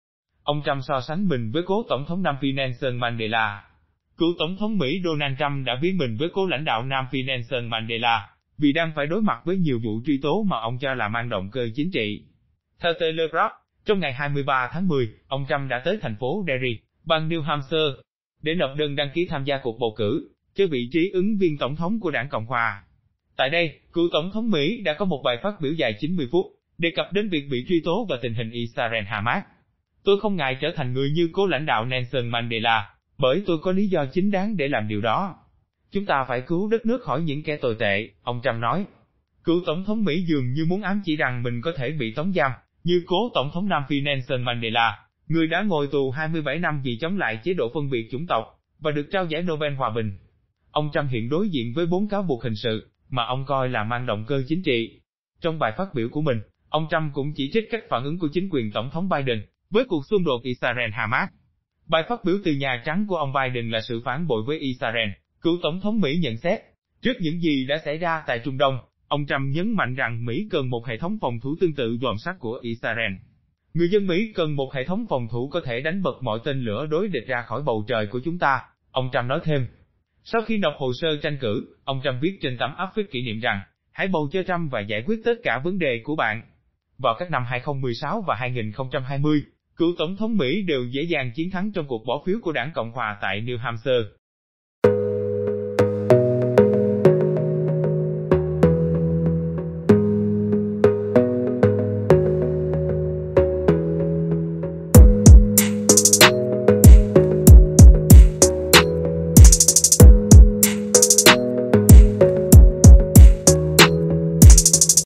Ông Trump so sánh mình với cố Tổng thống Nam Phi Nelson Mandela/Cựu Tổng Thống Mỹ Donal Trump
Cựu Tổng thống Mỹ Donald Trump đã ví mình với cố lãnh đạo Nam Phi Nelson Mandela vì đang phải đối mặt với nhiều vụ truy tố mà ông cho là mang động cơ chính trị.

Theo Telegraph, trong ngày 23/10, ông Trump đã tới thành phố Derry (bang New Hampshire) để nộp đơn đăng ký tham gia cuộc bầu cử cho vị trí ứng viên Tổng thống của đảng Cộng hòa. Tại đây, cựu Tổng thống Mỹ đã có một bài phát biểu dài 90 phút, đề cập đến việc bị truy tố và tình hình Israel-Hamas.

"Tôi không ngại trở thành người như cố lãnh đạo Nelson Mandela, bởi tôi có lý do chính đáng để làm điều đó. Chúng ta phải cứu đất nước khỏi những kẻ tồi tệ", ông Trump nói.

Cựu Tổng thống Mỹ dường như muốn ám chỉ rằng mình có thể bị tống giam như cố Tổng thống Nam Phi Nelson Mandela - người đã ngồi tù 27 năm vì chống lại chế độ phân biệt chủng tộc và được trao giải Nobel Hòa bình. Ông Trump hiện đối diện với 4 cáo buộc hình sự mà ông coi là mang động cơ chính trị.

Trong bài phát biểu của mình, ông Trump cũng chỉ trích cách phản ứng của chính quyền Tổng thống Biden với cuộc xung đột Israel-Hamas. "Bài phát biểu từ Nhà Trắng của ông Biden là sự phản bội với Israel", cựu Tổng thống Mỹ nhận xét.

Trước những gì đã xảy ra tại Trung Đông, ông Trump nhấn mạnh rằng Mỹ cần một hệ thống phòng thủ tương tự Vòm Sắt của Israel. "Người dân Mỹ cần một hệ thống phòng thủ có thể đánh bật mọi tên lửa đối địch ra khỏi bầu trời của chúng ta", ông Trump nói thêm.

Sau khi nộp hồ sơ tranh cử, ông Trump viết trên tấm áp phích kỷ niệm rằng: "Hãy bầu cho Trump và giải quyết tất cả vấn đề của bạn". Vào các năm 2016 và 2020, cựu Tổng thống Mỹ đều dễ dàng chiến thắng trong cuộc bỏ phiếu của đảng Cộng hòa tại New Hampshire.

Cập nhật liên tục các Nội Dung tin nóng đang được chú ý quan tâm.